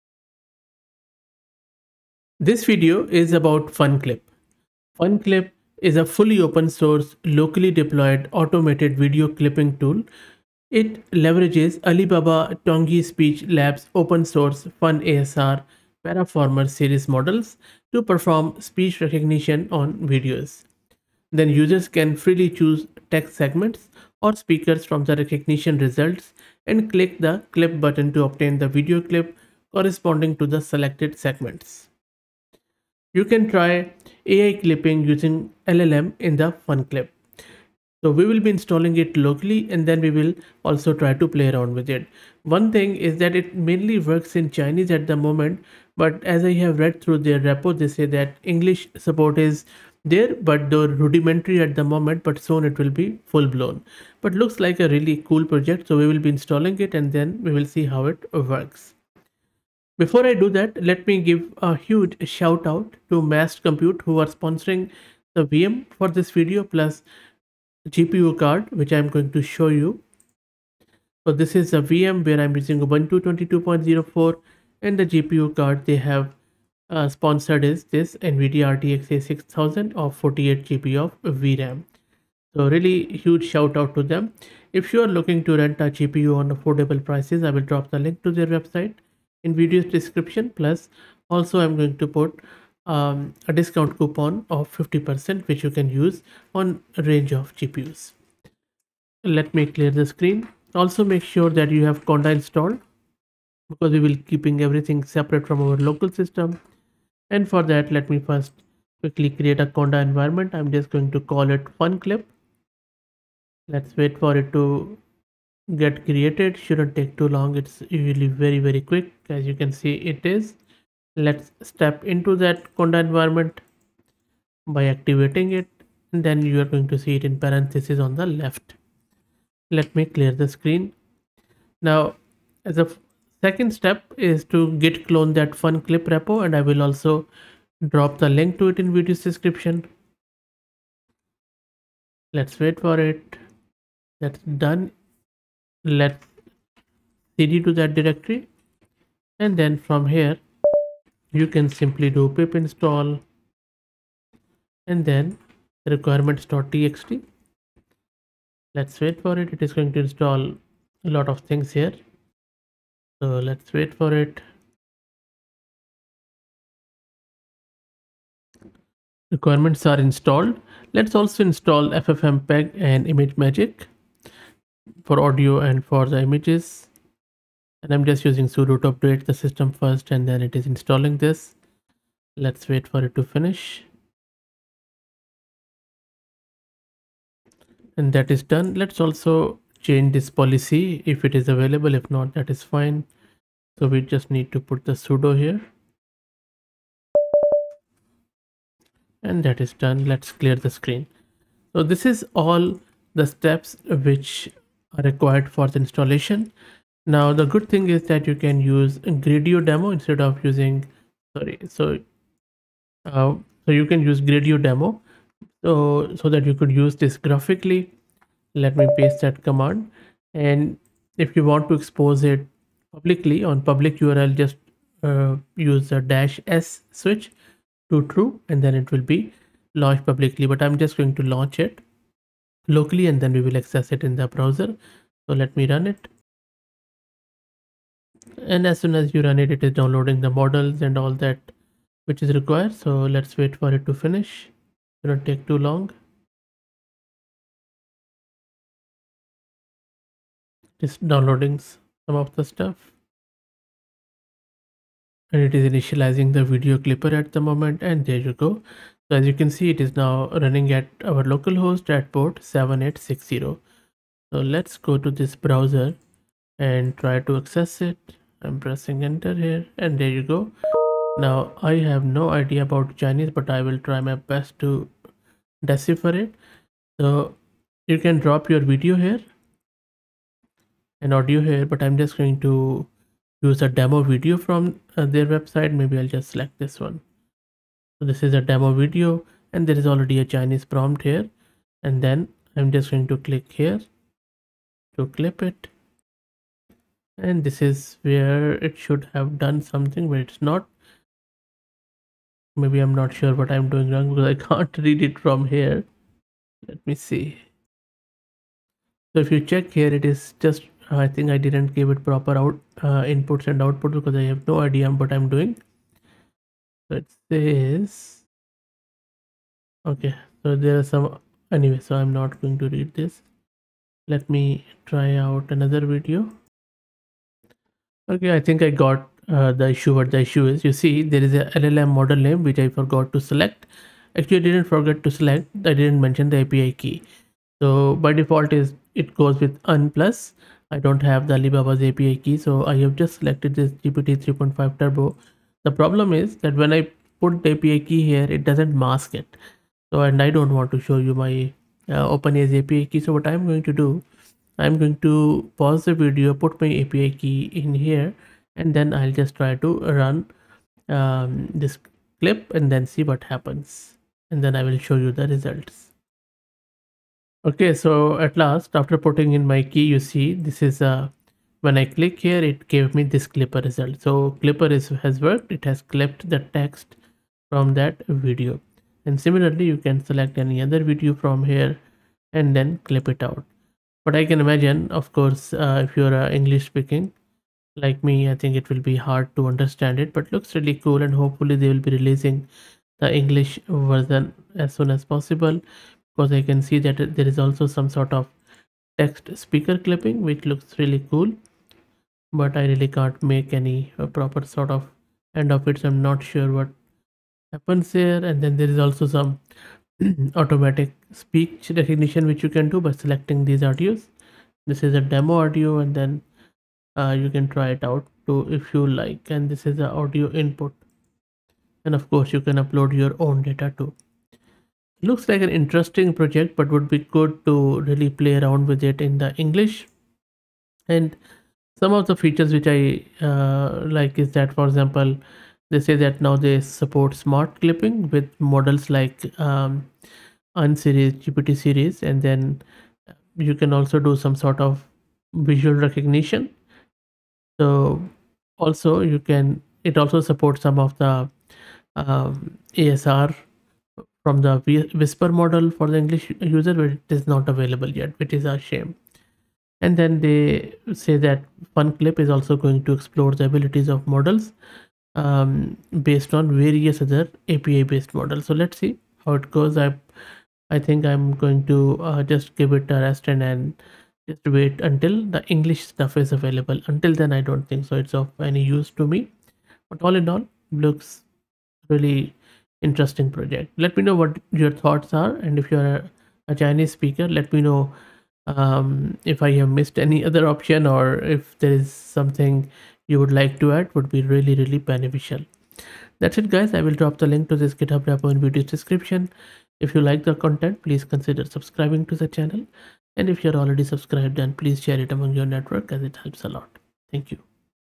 FunClip - AI Powered Video Clipping Tool
This video installs Alibaba's Funclip locally. It's a fully open-source, locally deployed automated video clipping tool. It leverages Alibaba TONGYI speech lab's open-source FunASR Paraformer series models to perform speech recognition on videos.